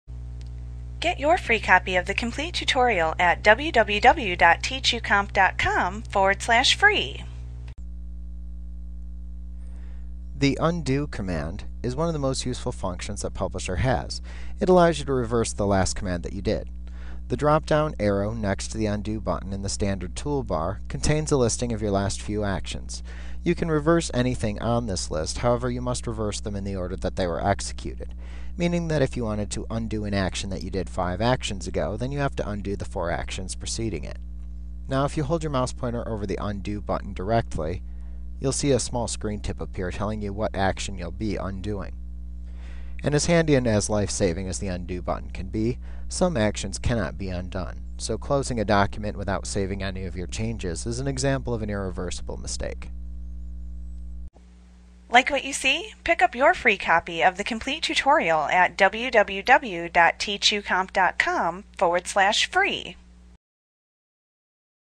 Publisher 2003 Tutorial The Undo Button Microsoft Training Lesson 6.9
Learn about the undo button in Microsoft Publisher - the most comprehensive Publisher tutorial available. Visit us today!